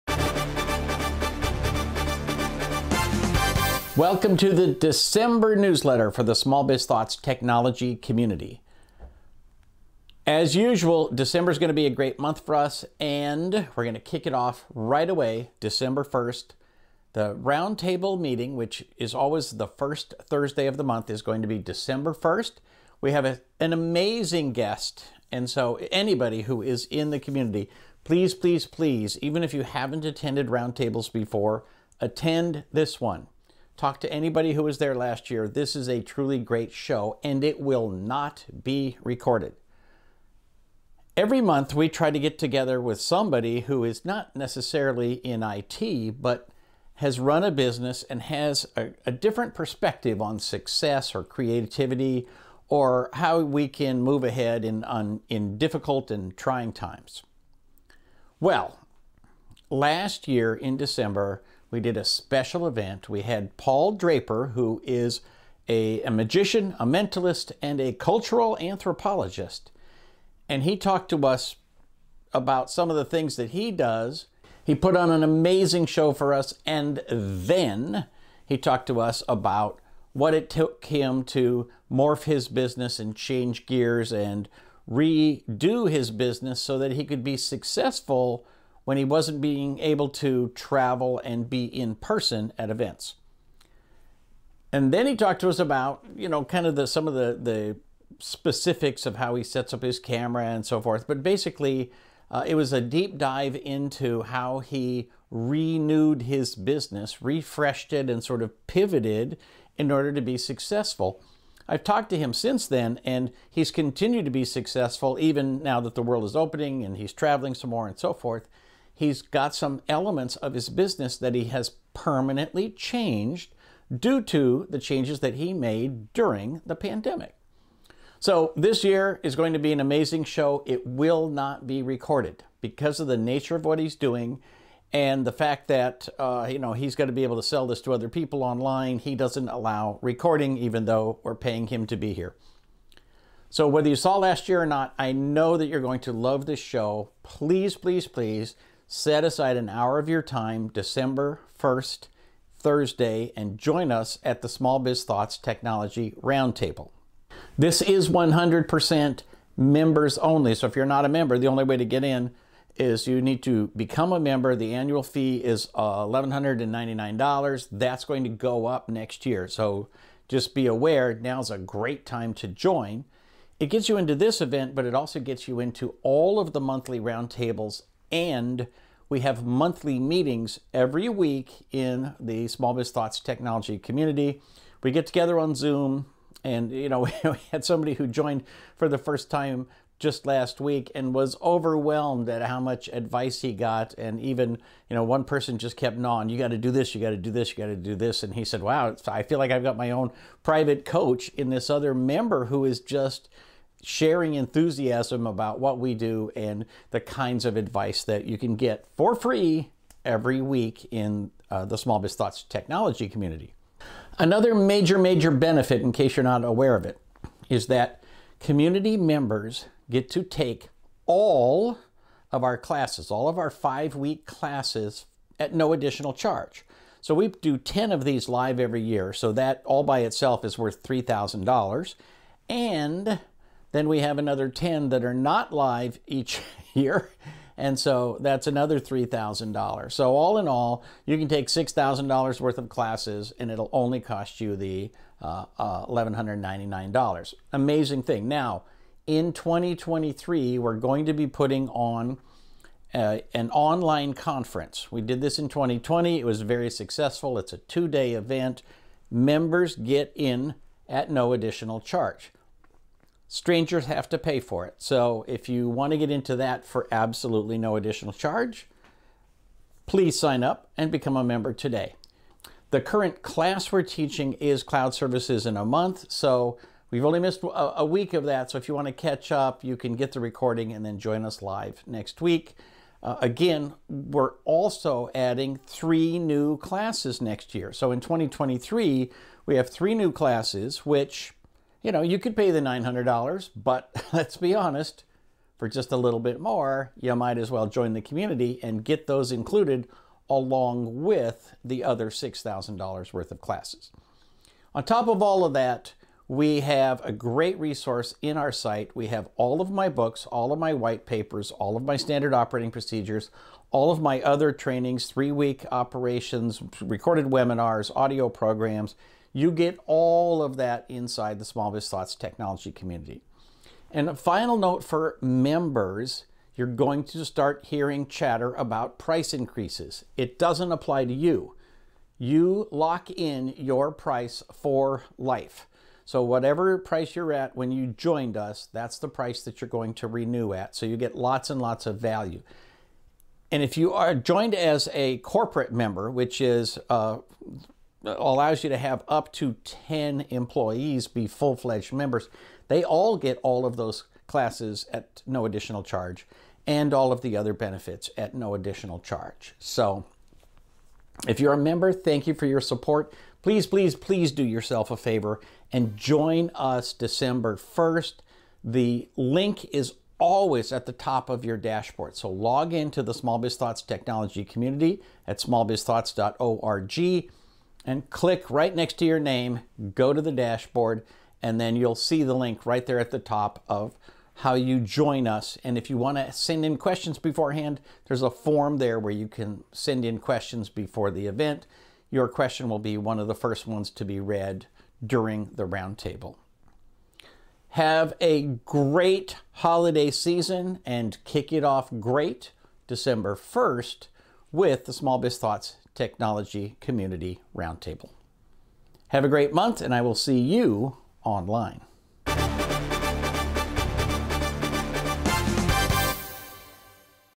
Community News - December 2022
SBT Community News - December 2022

Do not miss the December Roundtable!

Timeline -
00:00 December newsletter - Small Biz Thoughts Technology Community
01:03 Join Paul Draper, magician, mentalist, cultural anthropologist
02:08 This year is going to be an amazing show
02:53 Membership gets you into all of our classes
03:59 $6,000 worth of classes
05:08 You also get all of my books and other content
06:03 Do yourself a favor and join us December 1st
06:46 Call to Action